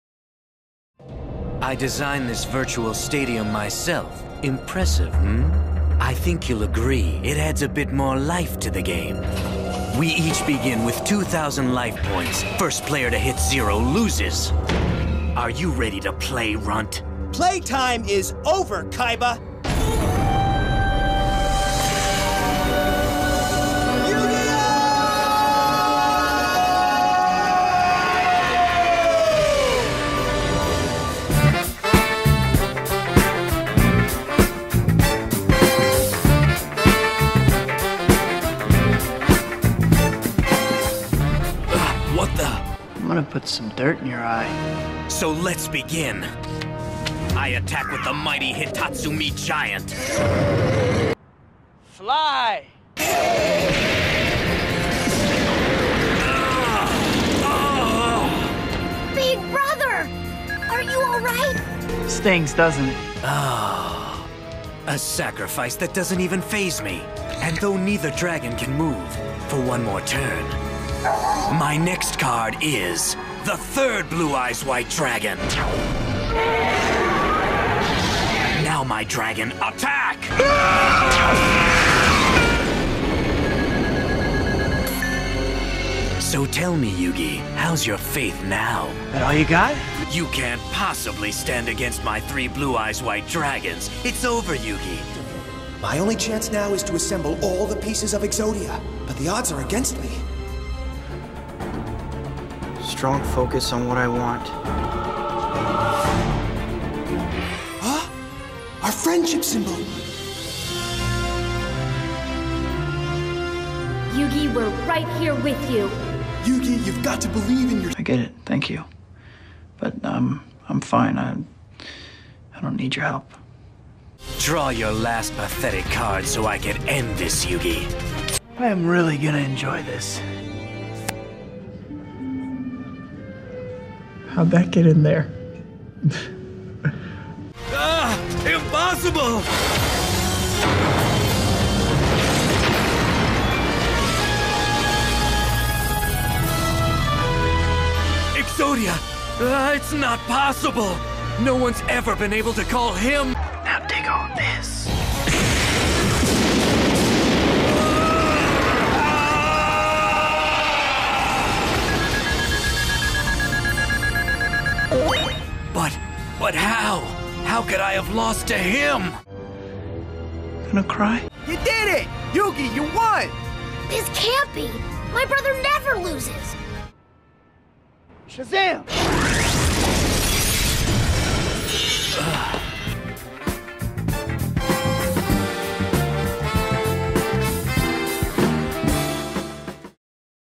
Bully Maguire vs Seto Kaiba (re-upload)
Hey everyone!

So YouTube locked me out of my original account and (for some unknown reason) wouldn't let me do account recovery questions. I had to restart 5kids Entertainment here and I'm posting this video as a re-upload for my new channel.

Thanks to Matan Animation Studios and T0bey for their excellent green screens.